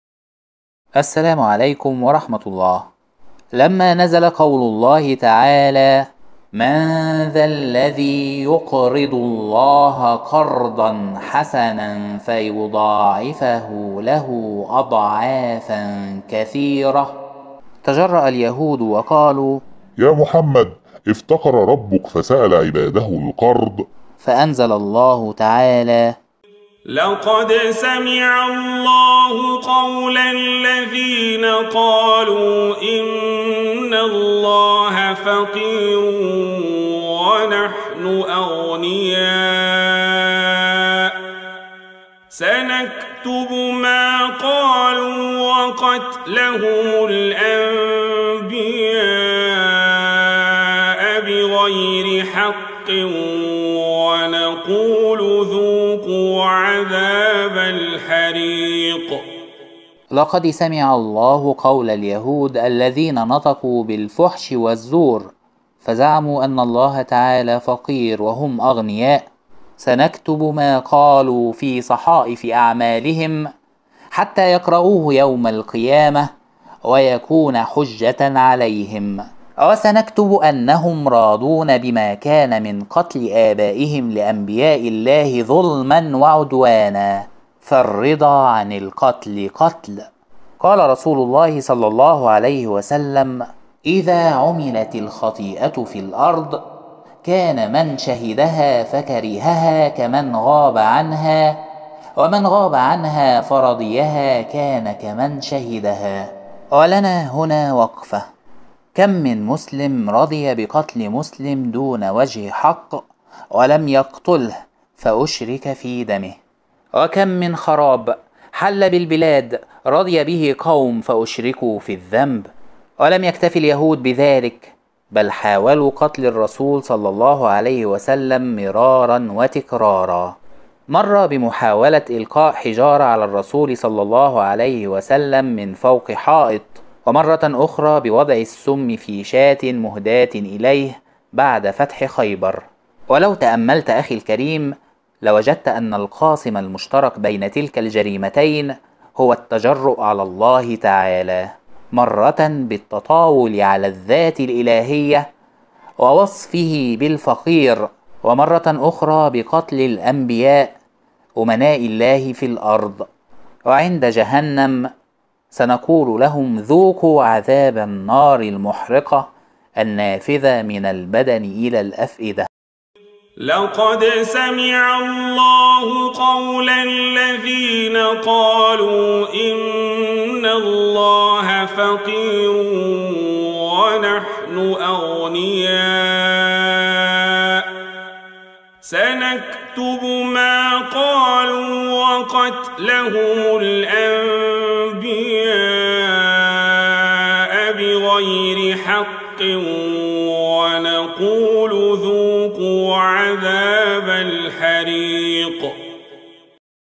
تفسير سورة آل عمران ١٨١: لقد سمع الله قول الذين قالوا إن الله فقير ونحن أغنياء ۘ...
لَّقَدْ سَمِعَ ٱللَّهُ قَوْلَ ٱلَّذِينَ قَالُوٓاْ إِنَّ ٱللَّهَ فَقِيرٌ وَنَحْنُ أَغْنِيَآءُ ۘ سَنَكْتُبُ مَا قَالُواْ وَقَتْلَهُمُ ٱلْأَنۢبِيَآءَ بِغَيْرِ حَقٍّۢ وَنَقُولُ ذُوقُواْ عَذَابَ ٱلْحَرِيقِ

التفسير الميسر : لقد سمع الله قول اليهود الذين قالوا: إن الله فقير إلينا يطلب منا أن نقرضه أموالا ونحن أغنياء. سنكتب هذا القول الذي قالوه، وسنكتب أنهم راضون بما كان مِن قَتْل آبائهم لأنبياء الله ظلمًا وعدوانًا، وسوف نؤاخذهم بذلك في الآخرة، ونقول لهم وهم في النار يعذبون: ذوقوا عذاب النار المحرقة.